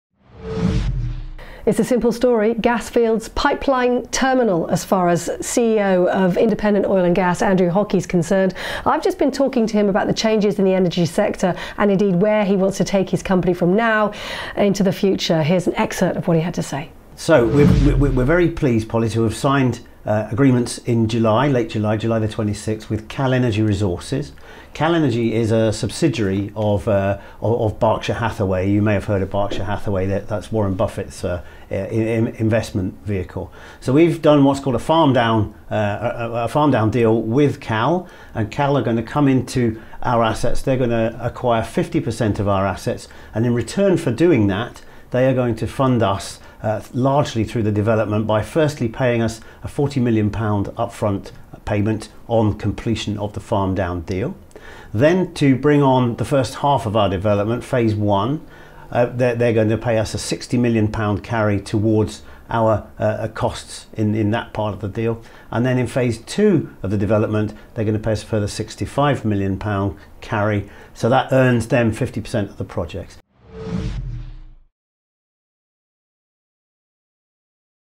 Independent Oil & Gas outlines company ambitions
Independent Oil and Gas (LON:IOG) tells Proactive Investors London where the company wants to position itself in the sector and why.
Chief Executive Andrew Hockey explains how the Phase 1 and Phase 2 development projects the company has been concentrating on are progressing, and provides news on the Harvey well.